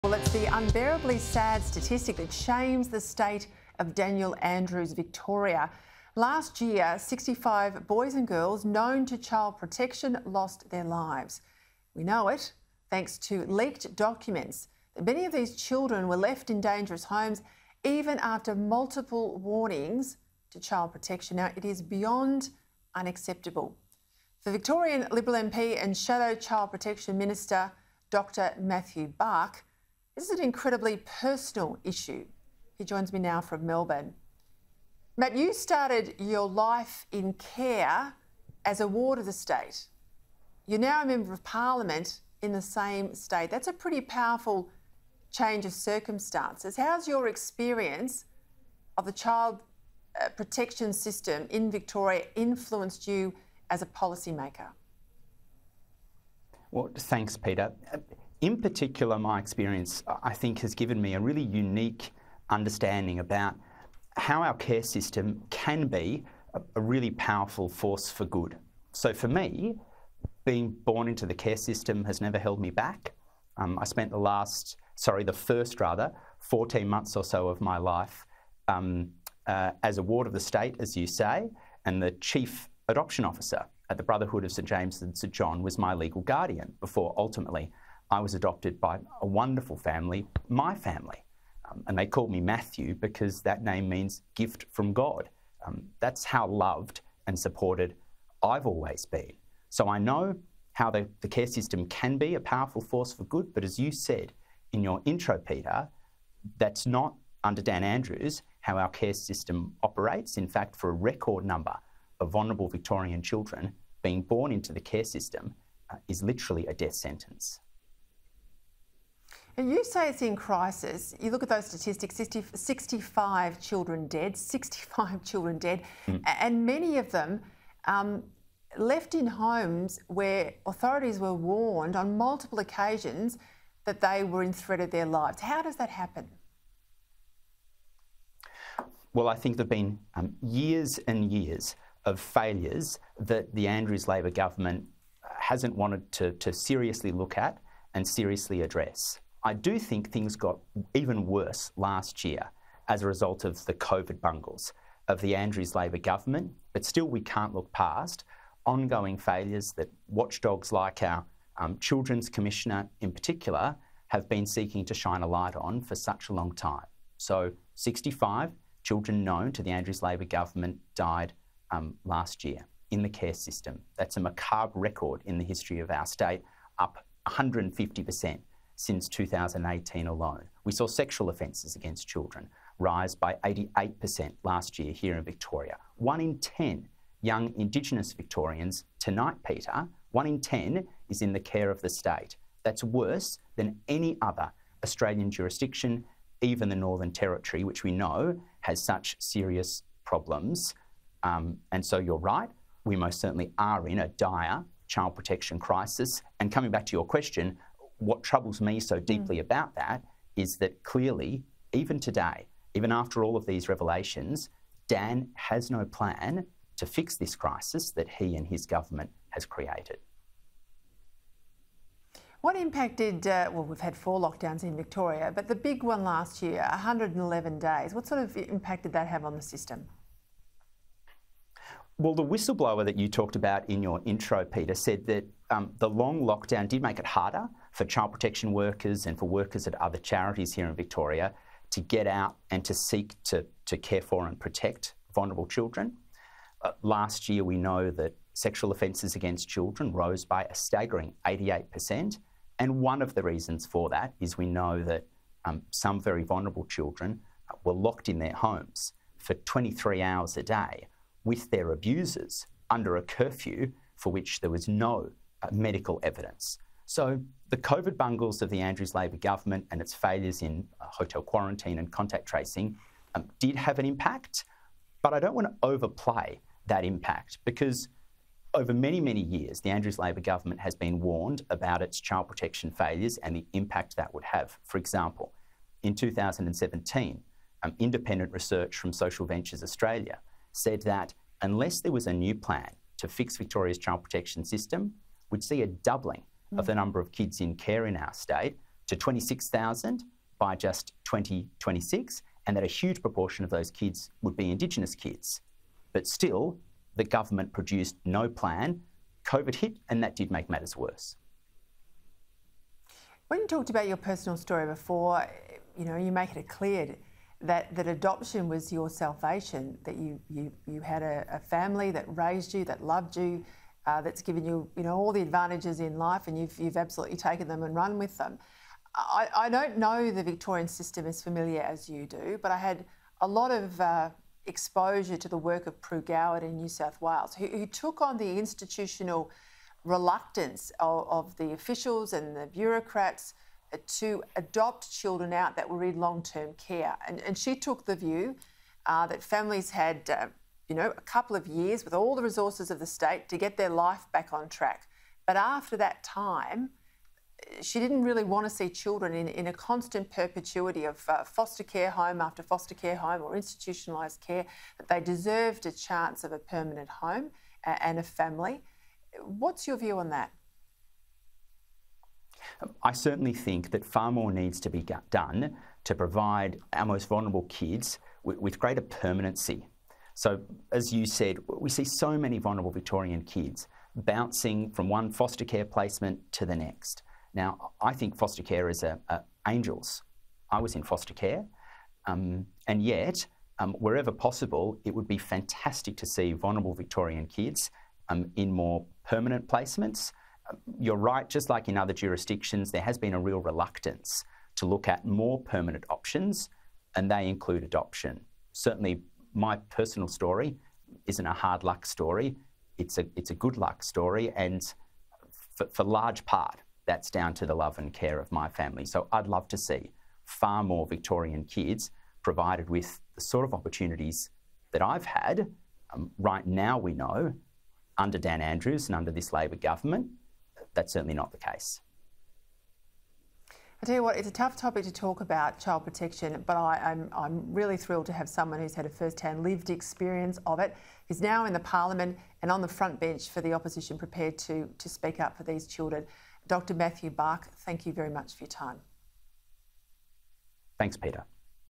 Vulnerable Victorian children born into ‘death sentence’ state care system: Liberal MP
For a number of vulnerable Victorian children, being born into the state’s care system is “literally a death sentence,” according to Victorian Liberal MP Dr Matthew Bach.

It comes amid news 65 boys and girls known to Victoria's Child Protection lost their lives in 2020.

Leaked documents further revealed many of those children were left in dangerous homes even after multiple warnings to Child Protection.

“I know how the care system can be a powerful force for good,” Dr Bach told Sky News Peta Credlin.

“But that’s not – under Dan Andrews – how our care system operates.”